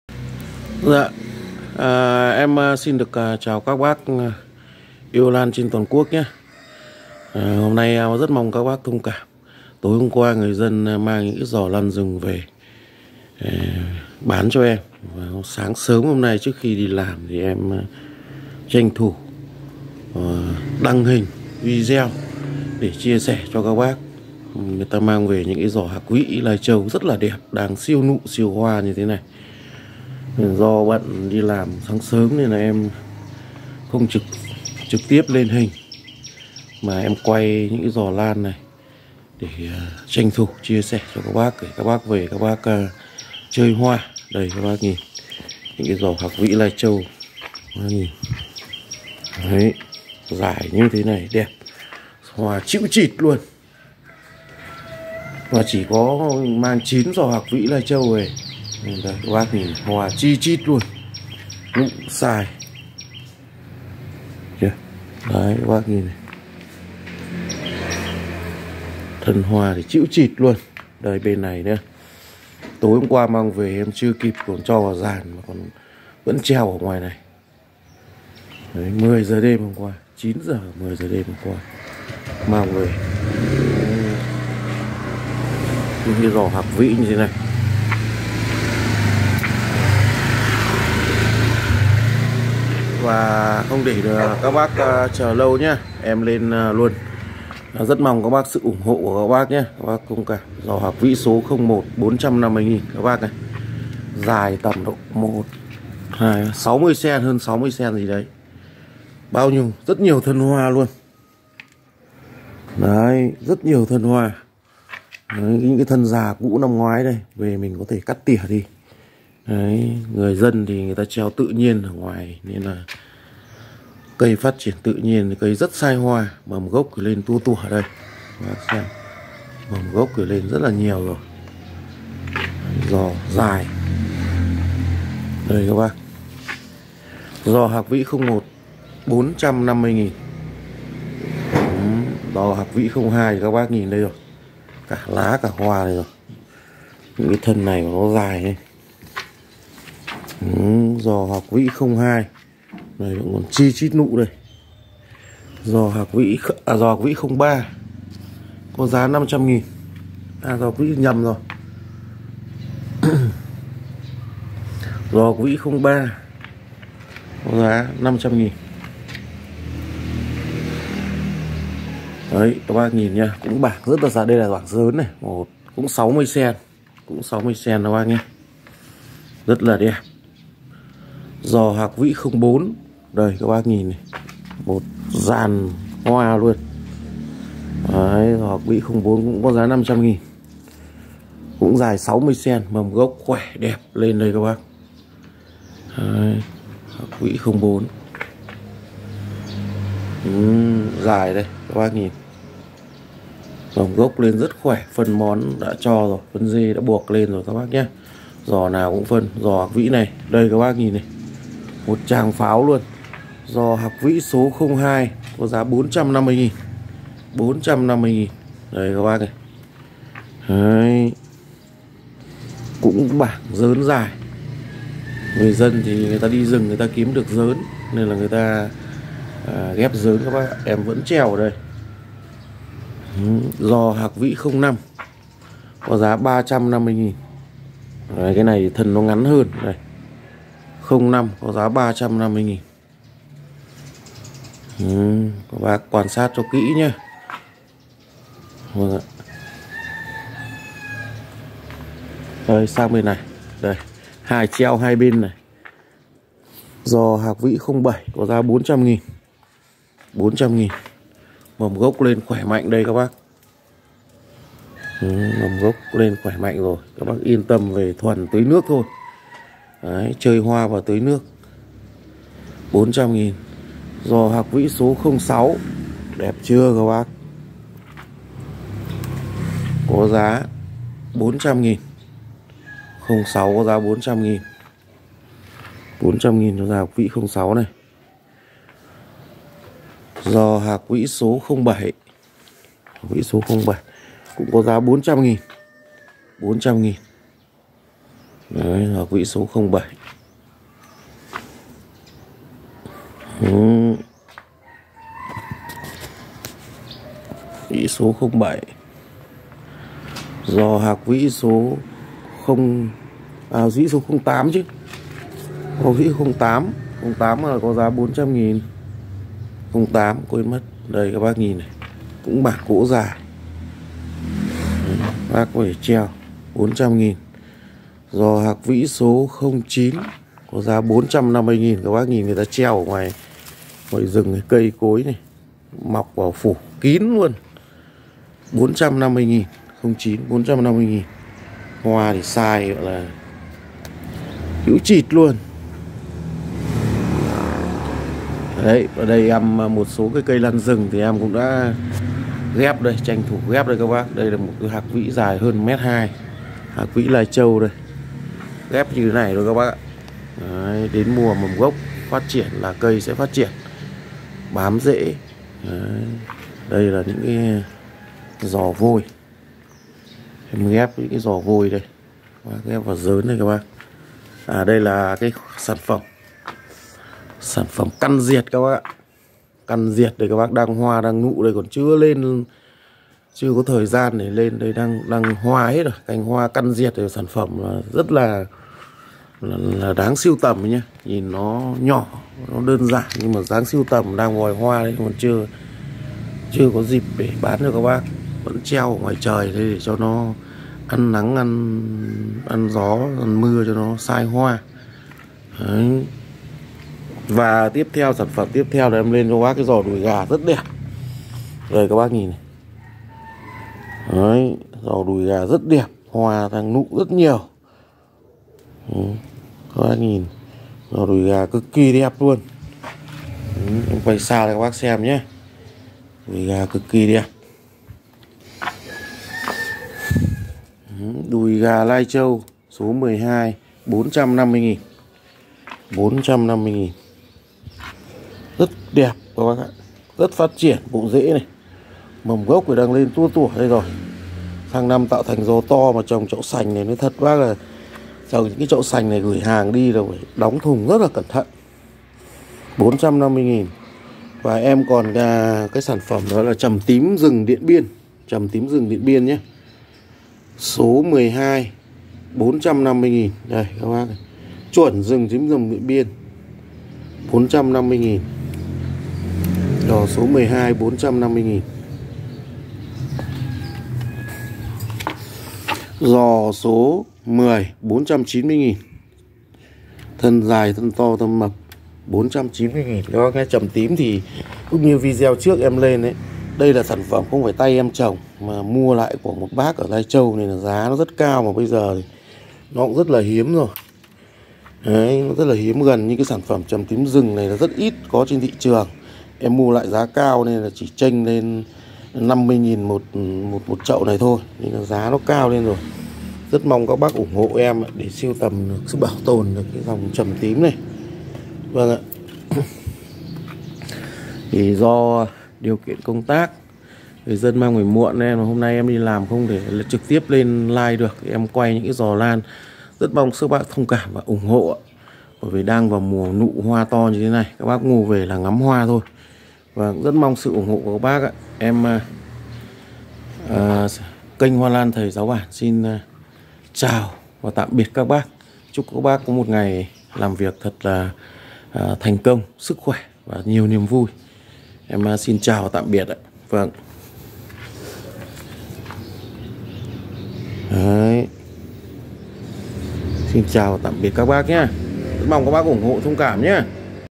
15/3 Người dân mang nhiều dò hạc vỹ về muộn Thầy Giáo Bản mong các bác ủng hộ 0868383345
Rất nhiều dò hạc vỹ người Mông thuần siêu nụ siêu hoa